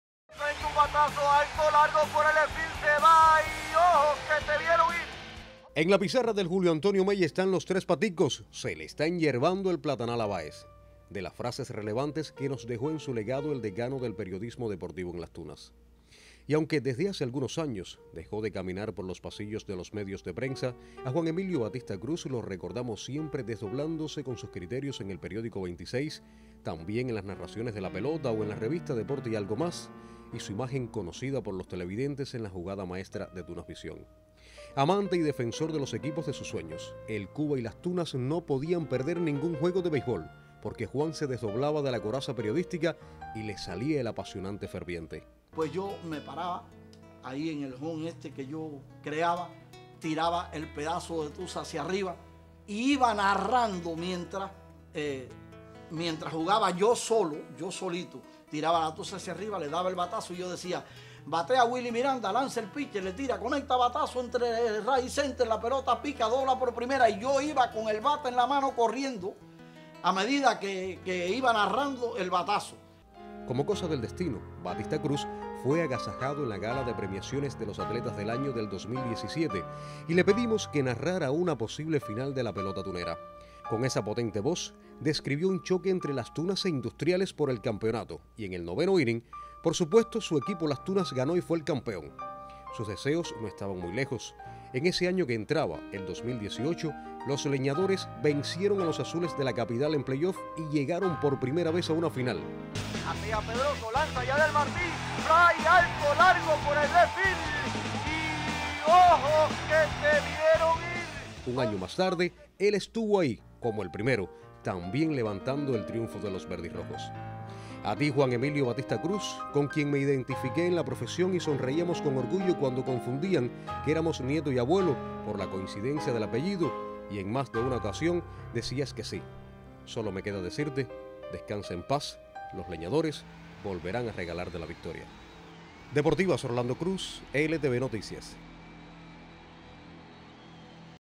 Fallece en LasTunas decano del periodismo deportivo Juan Emilio Batista Cruz
El periodismo deportivo tunero está de luto. El destacadísimo narrador y comentarista deportivo Juan Emilio Batista Cruz, falleció hoy a la edad de 79 años.